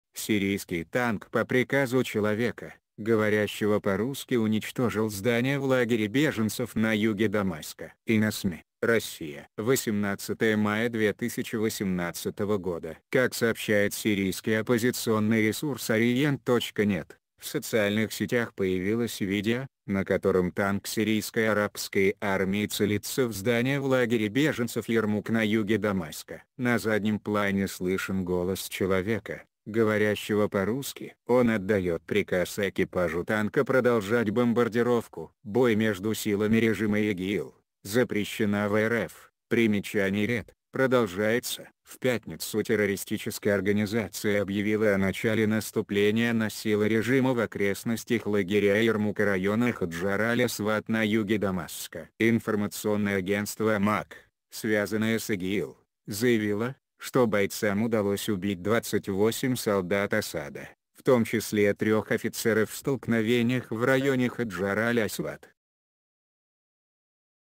Сирийский танк по приказу «человека, говорящего по-русски» уничтожил здание в лагере беженцев на юге
Сирийский танк по приказу «человека, говорящего по-русски» уничтожил здание в лагере беженцев на юге Дамаска.
ИноСМИ, Россия.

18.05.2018

На заднем плане слышен голос человека, говорящего по-русски. Он отдает приказ экипажу танка продолжать бомбардировку.    Бои между силами режима и ИГИЛ ( запрещена в РФ — прим. ред .) продолжаются. В пятницу террористическая организация объявила о начале наступления на силы режима в окрестностях лагеря Ярмук и района Хаджар Аль-Асвад на юге Дамаска. Информационное агентство «Амак», связанное с ИГИЛ, заявило, что бойцам удалось убить 28 солдат Асада, в том числе трех офицеров в столкновениях в районе Хаджар Аль-Асвад.  ...